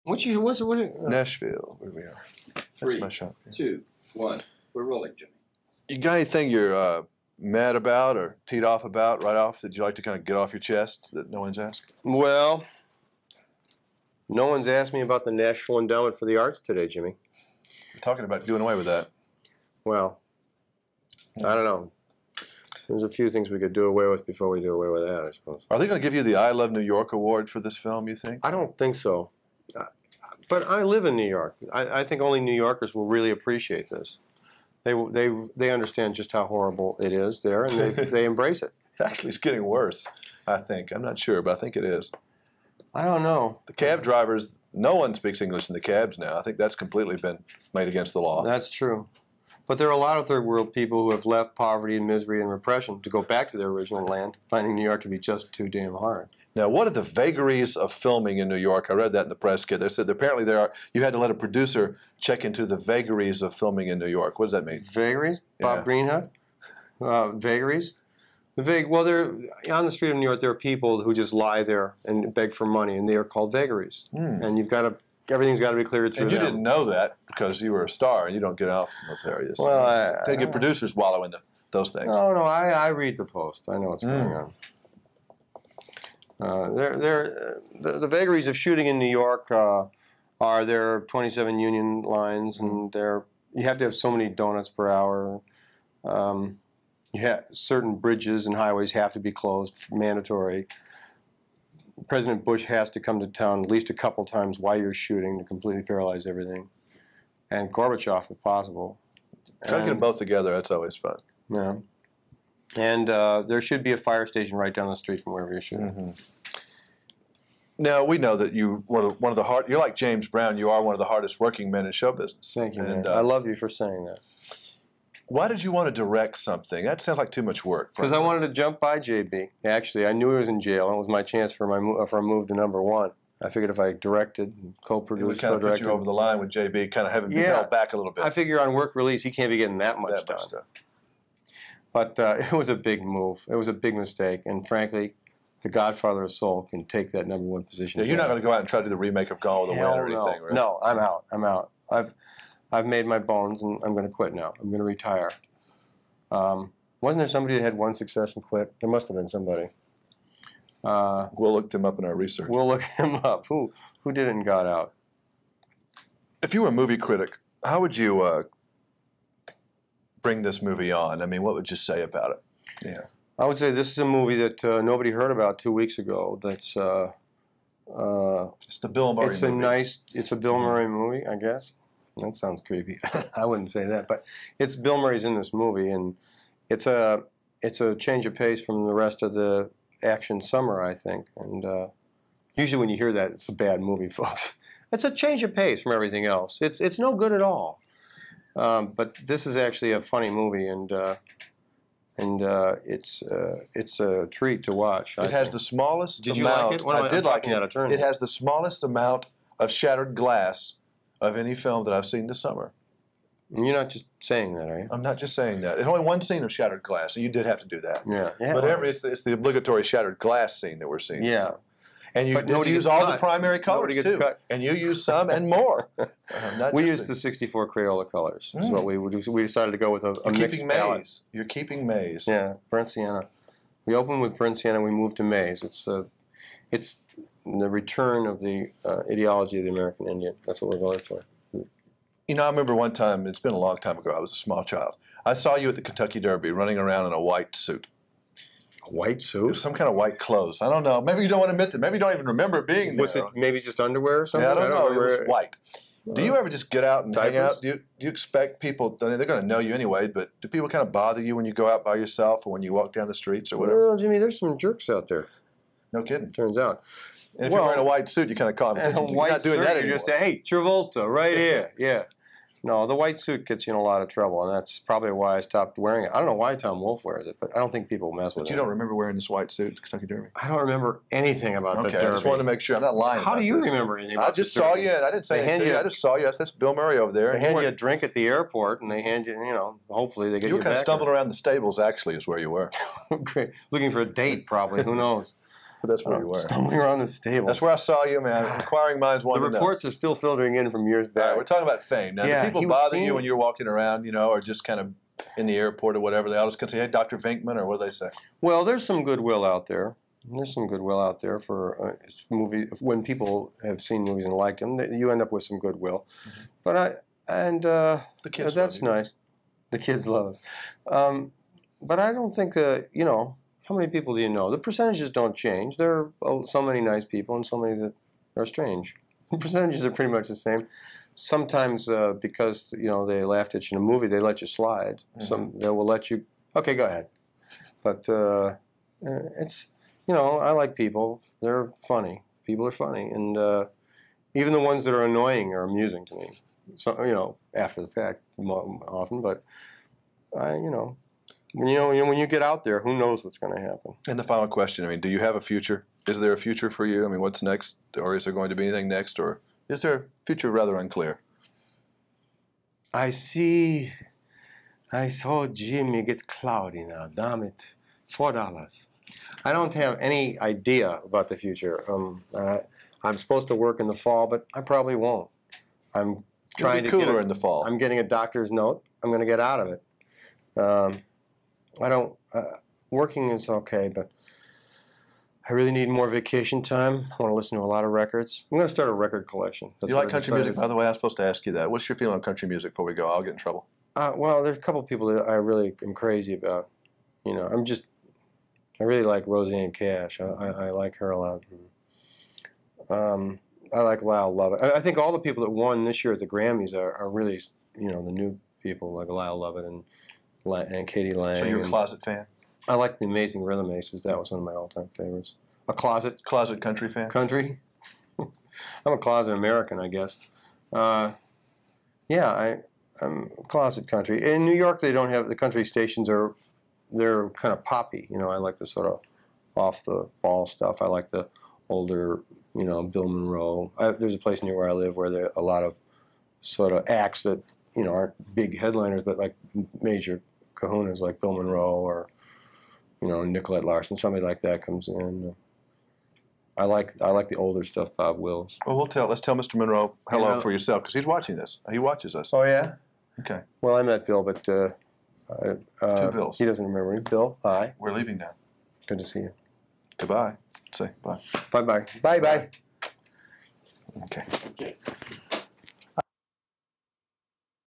Bill Murray and Jimmy Carter in NYC HD 720p
I have always thought Bill Murray was one of the great comic forces of the last 50 years...SNL to Stripes..Ghostbusters and so many more...Interviewed him regularly for over 15 years. this time a movie he directed Quick Change